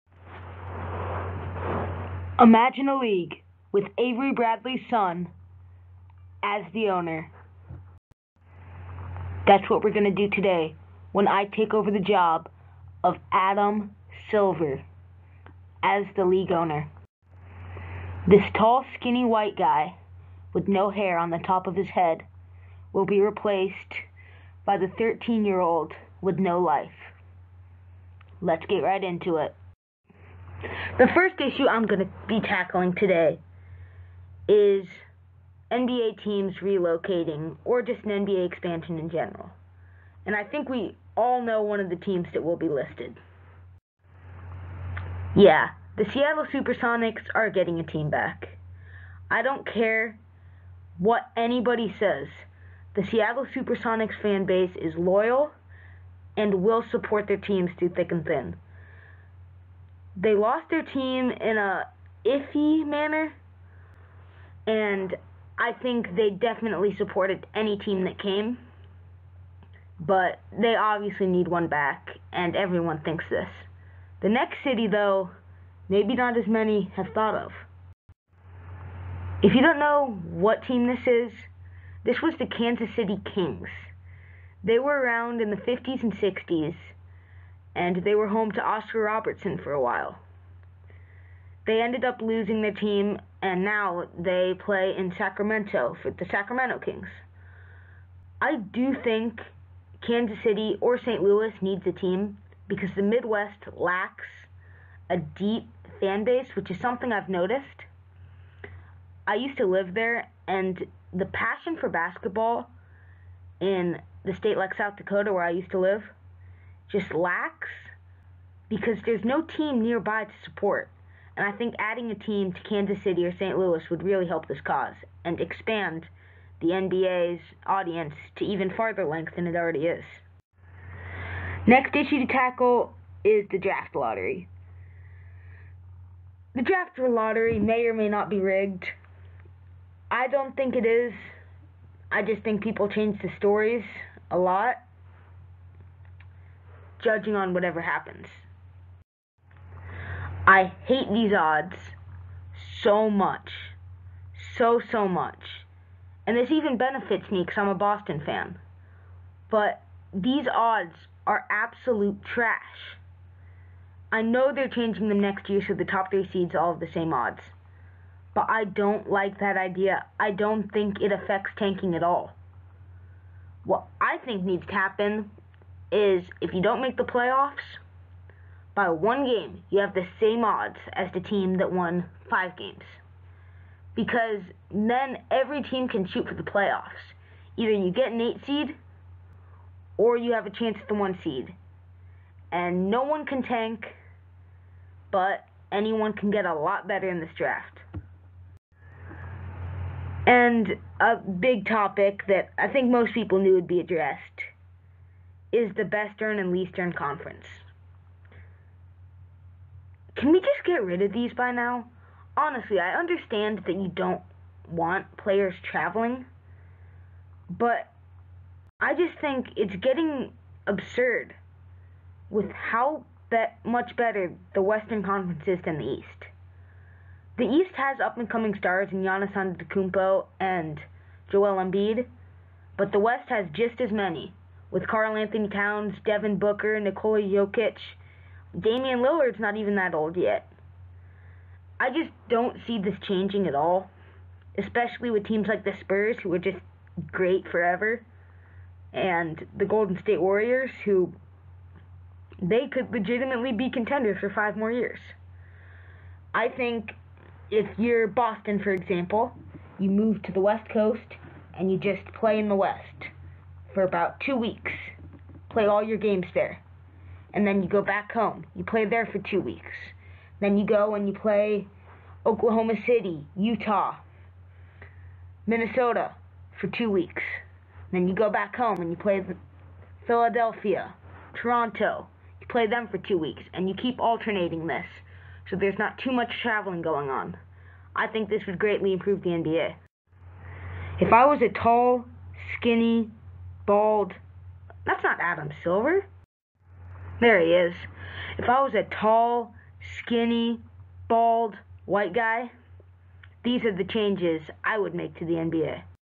If I Was Adam Silver...
If I was Adam Silver the league wouldn't be good anymore... it would be great again. Shoutout to King Benny Garcia and MDJ refuses to put me in his livestreams so screw him. I will have an Instagram and Twitter soon